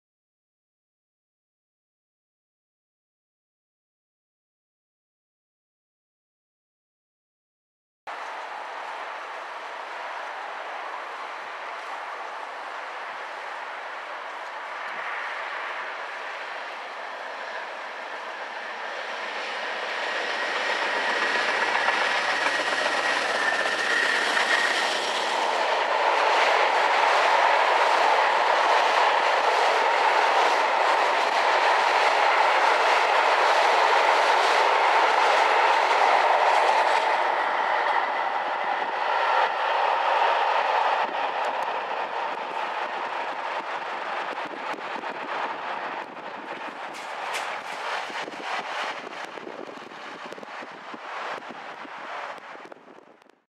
Colas 56087. 6J37. Logs. Hest Bank. 17/10/13
Video of Colas 56087 with 6J37 Carlisle Yd to Chirk, Loaded Logs.
Filmed passing Hest Bank on 17/10/13.